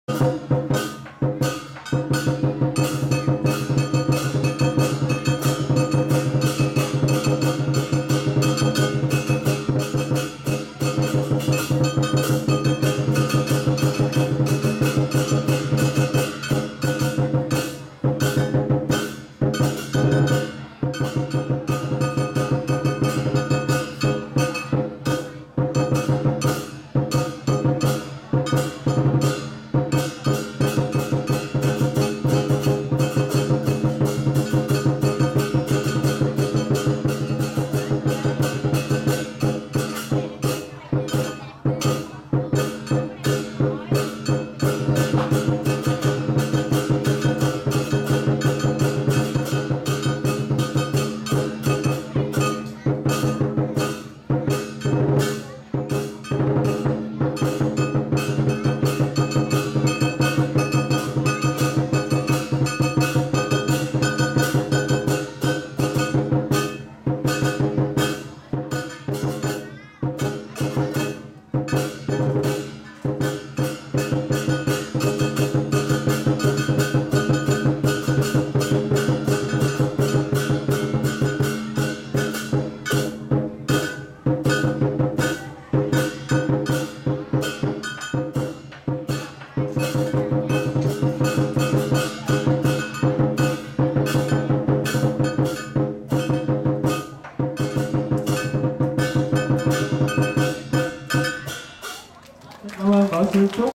Song Kiếm Tam Bộ Tuyết Hoa
Lể kỹ niệm 27 năm (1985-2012) ngày mất của Sư Trưởng Trương Thanh Đăng tại bến Lức Long An ngày 16/09/2012